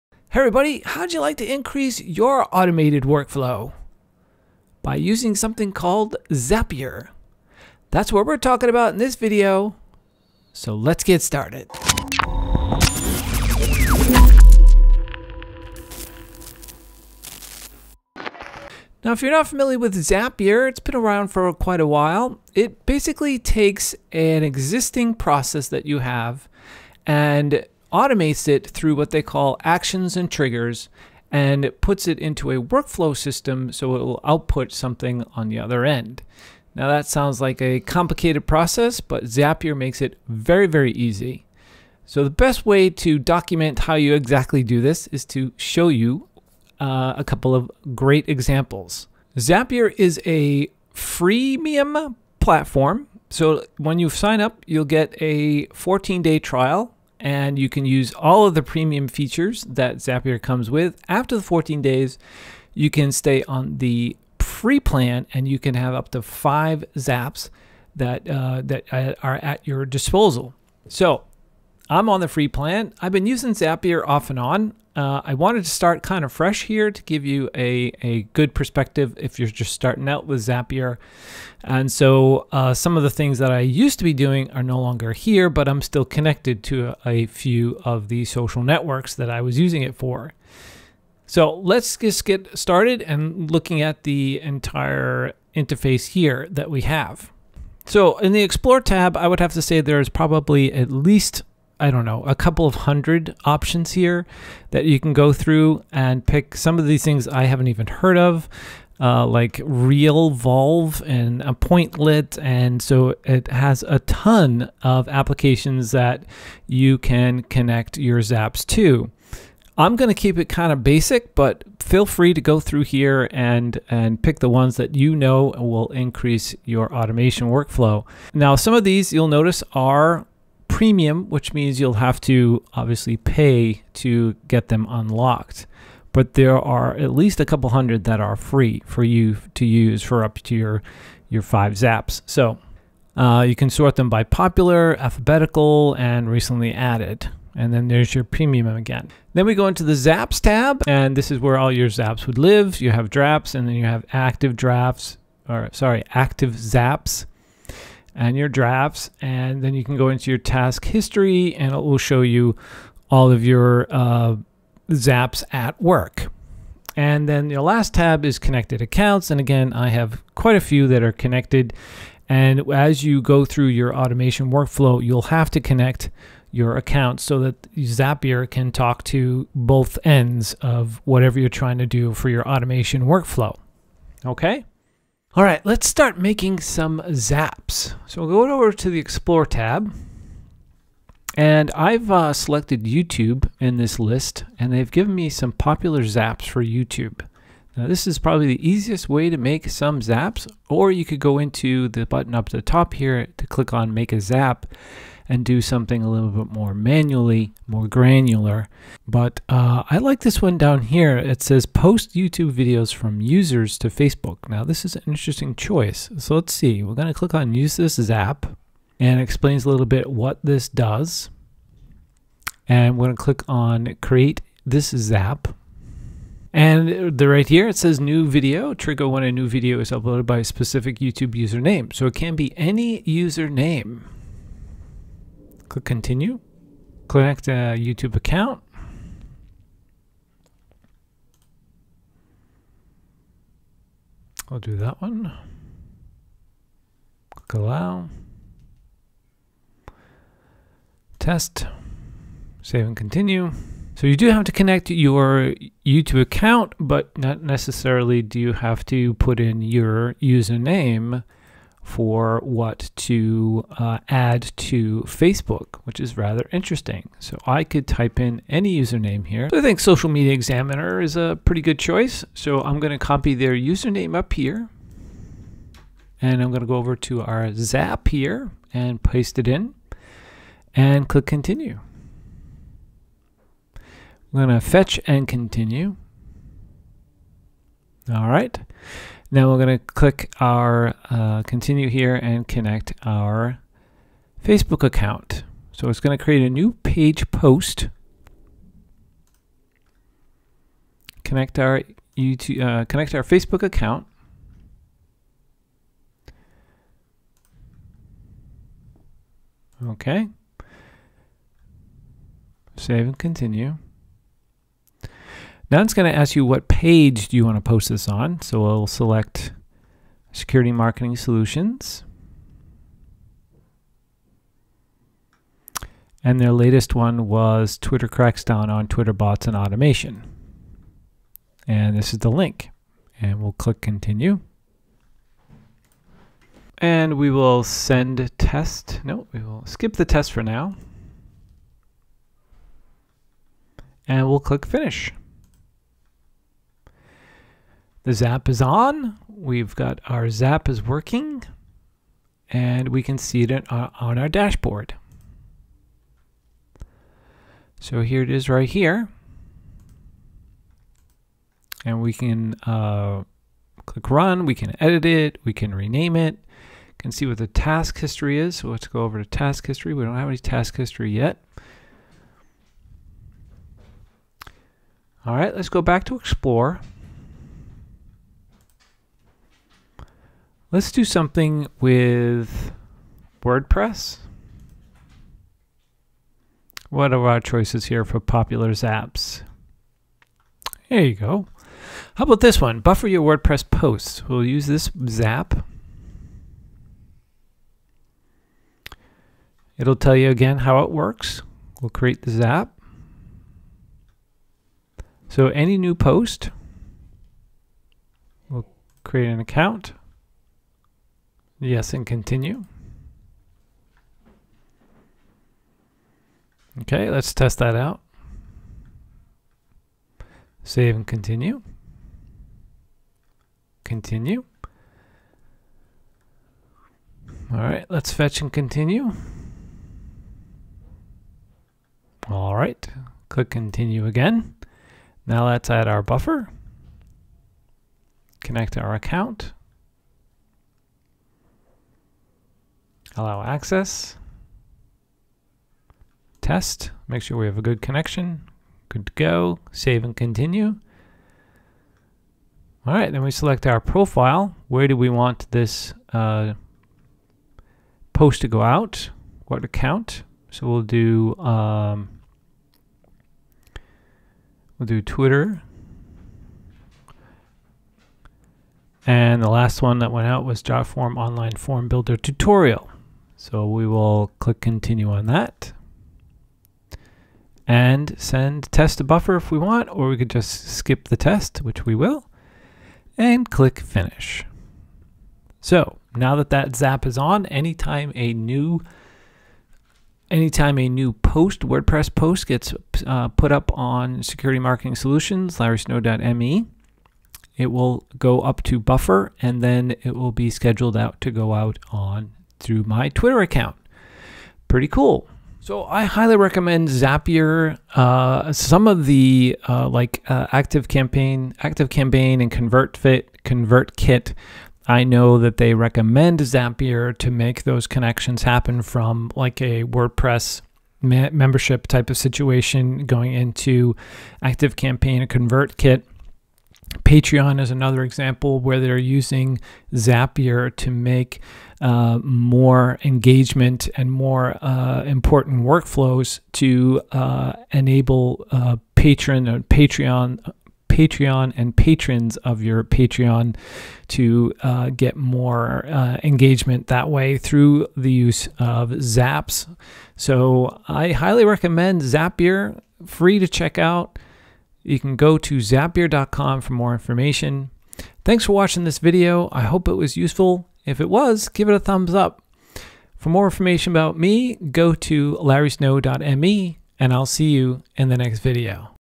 Zapier tutorial: Workflow Automation at Your Fingertips
In this video tutorial, I guide you through Zapier, an online workflow automation tool. Zapier is the easiest way to automate your day-to-day tasks and save time. Connect your favorite tool and create a workflow to perform a repetitive task automatically.

Start setting up your Zaps and increase your productivity with the time you save.

And you can find more great articles and tutorials on Facebook and other social media networks on my blog at